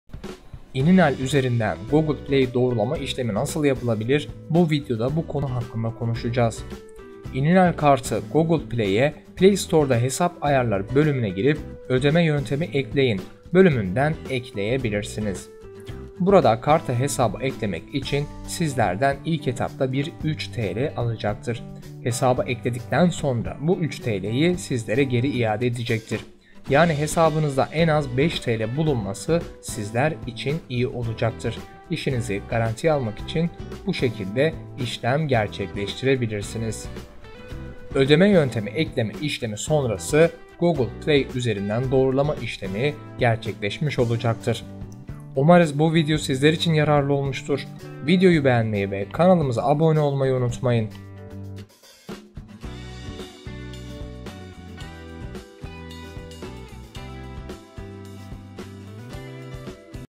İninal Google Play Doğrulama Nasıl Yapılır
Bu videoda ininal doğrulama nasıl yapılır,
ininal google play doğrulama gibi merak edilen sorulara sizler için yanıt verdik.